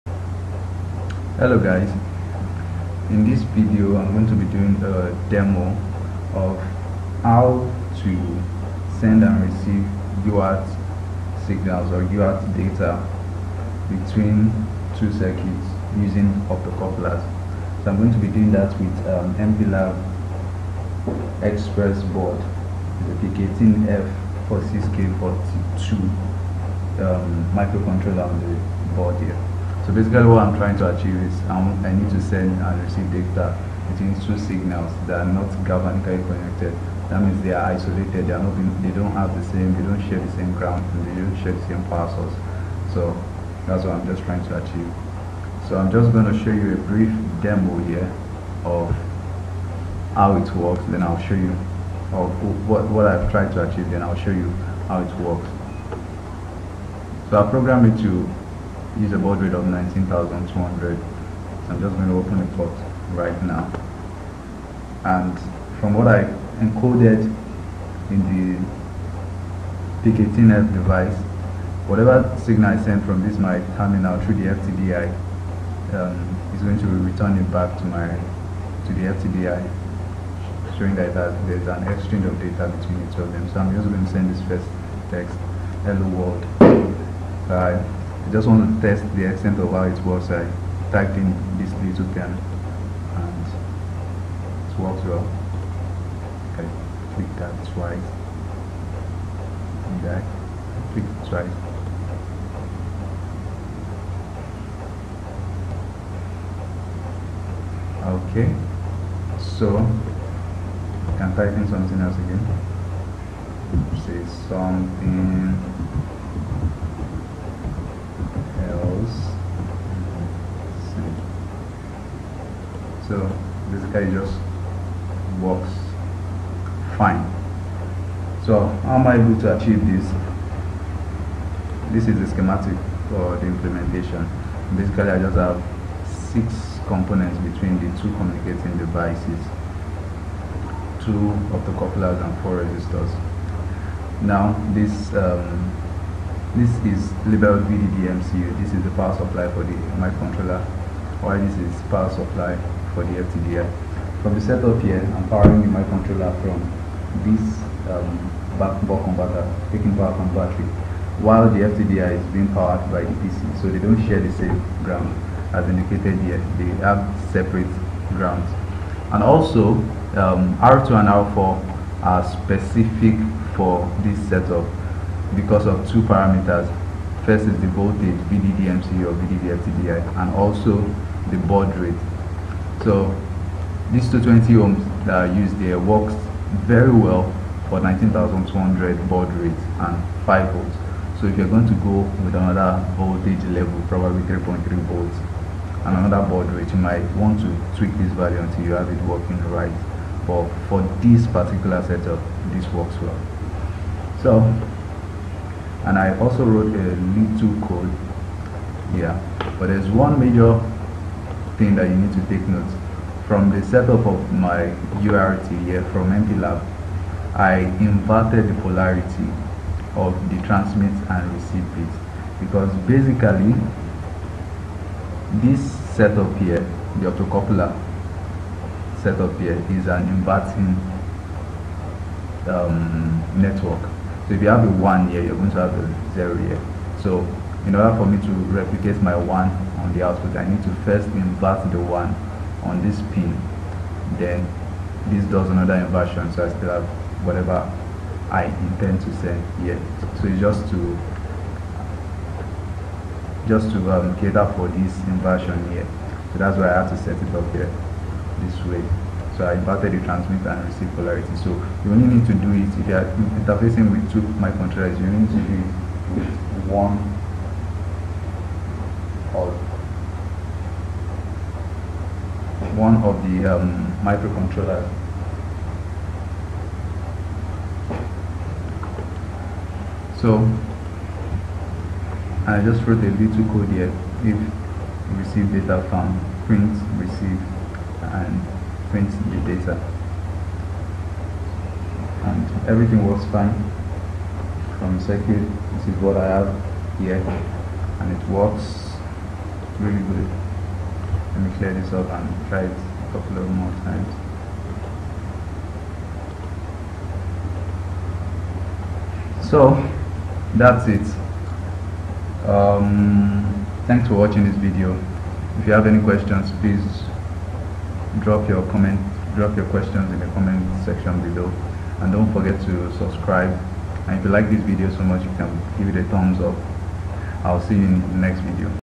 How to implement UART communications via PC817 optocouplers using PIC18F46k42 and FTDI
How to implement Serial communications (UART) through PC817 optocouplers. This is applicable for applications that require transmission and reception of data over UART between two circuits that are galvanically isolated or between two circuits that do not have a common ground point even if they're not galvanically isolated. This Demo works with baud rates of 19200 although it can be used for lower baud rate, but not higher. I hope this is helpful.